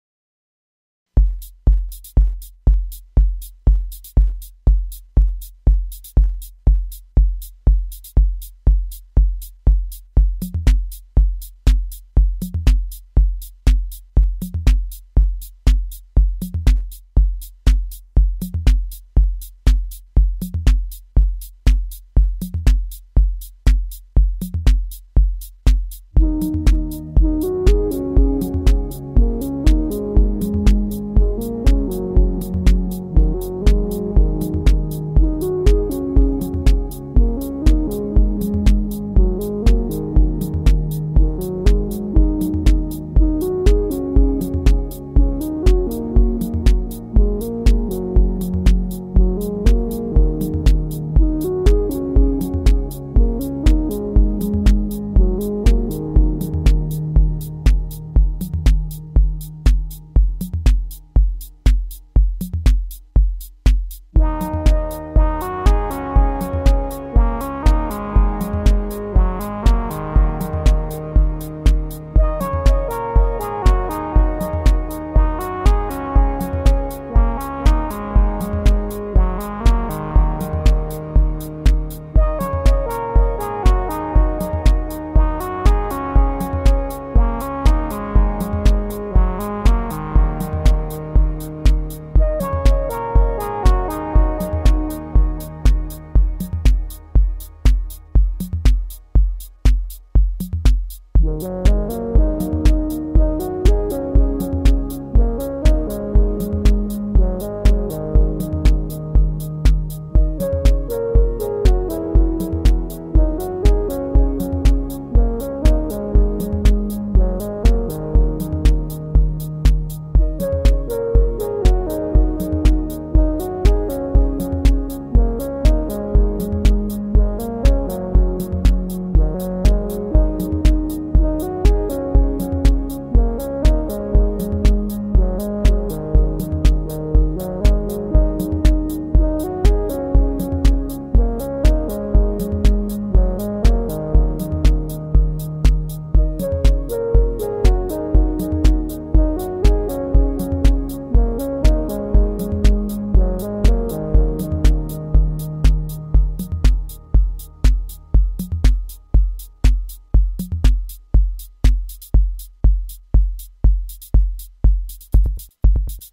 Volca Beats - MS20 - Erica Synths Delay
Playing with the Korg Volca Beats and the MS-20 through the freshly built Erica Synths Delay module